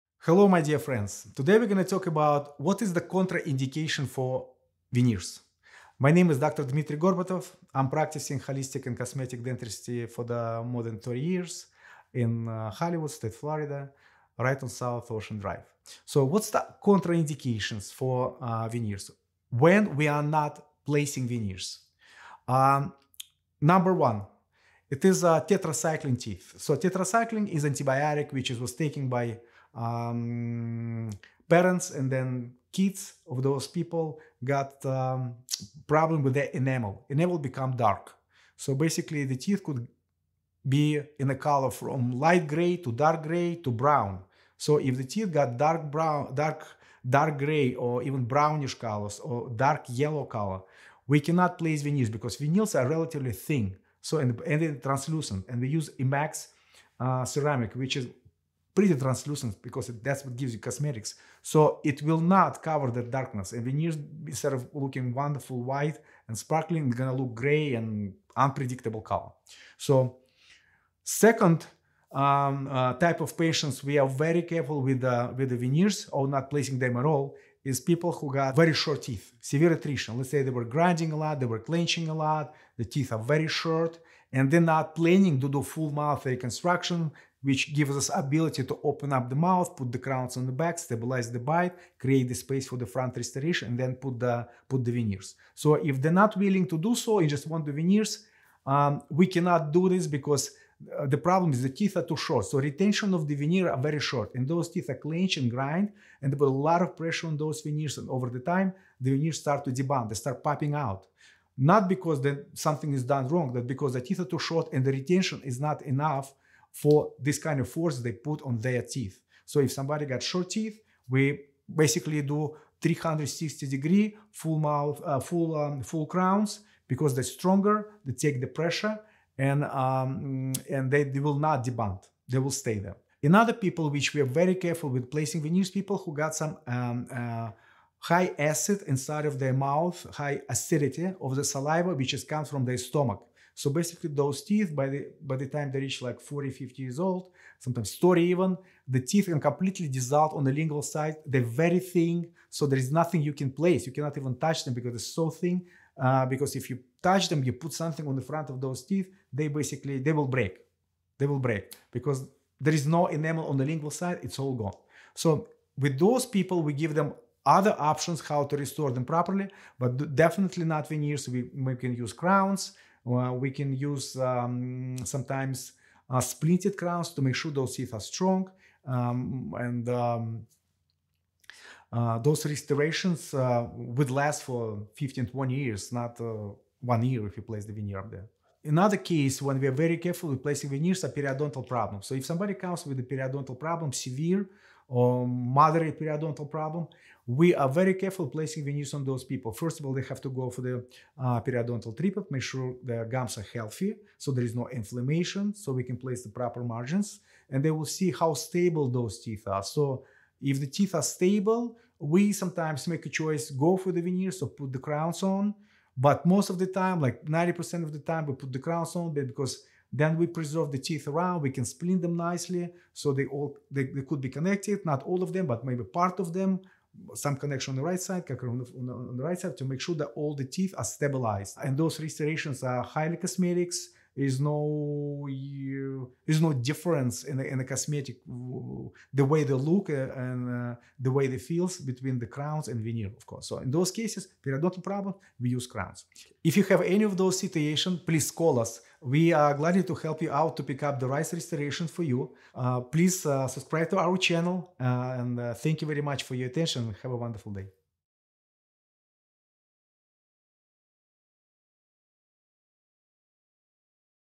Contraindications for Veneers: When Not to Choose Veneers
In this essential video guide, Dr. Dmitry Gorbatov, with over 30 years of experience in holistic and cosmetic dentistry, addresses a vital topic often overlooked in cosmetic dentistry discussions: the contraindications for dental veneers. Broadcasting from his well-established practice in Hollywood, Florida, Dr. Gorbatov delves deep into the scenarios where veneers might not be the best solution for dental restoration, offering his profound insights and expert knowledge.

Discover why certain conditions, such as teeth darkened by tetracycline or extensive physical wear, might not be suitable for the traditional veneer approach. Dr. Gorbatov explains the technical limitations of veneers, particularly in cases of severe enamel discoloration, short teeth due to attrition, high saliva acidity, and existing periodontal diseases. He emphasizes the importance of choosing the right dental restoration method that not only enhances cosmetic appearance but also preserves the long-term health and functionality of your teeth.

This video is an invaluable resource for anyone considering veneers or seeking alternative solutions for dental restoration. Whether you're a patient faced with these dental challenges or a dental professional seeking to expand your knowledge, Dr. Gorbatov's insights will guide you towards making informed, health-first dental decisions.

Gorbatov Dentistry⠀
Mon, Tue, Wed, Thu, Fri 9 am - 5 pm,  Sat - By Appointment⠀
DG Dentistry Inc.